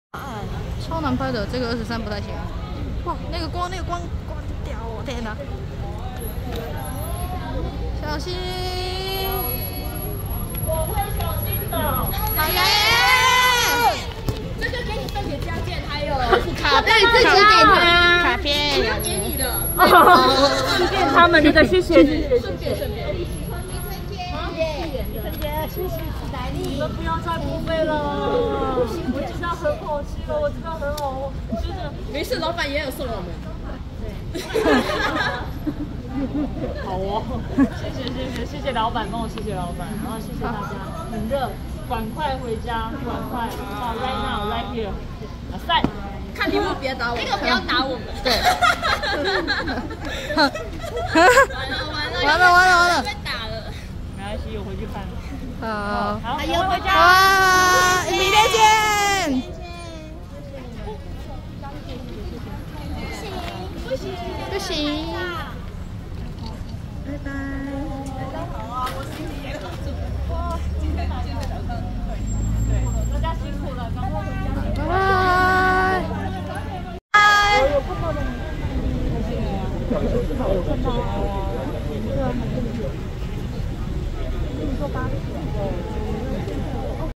20230610 同學會同鞋台中場 賴雅妍 Megan Lai 下班
中間我在把卡片給雅妍，所以有點沒拍到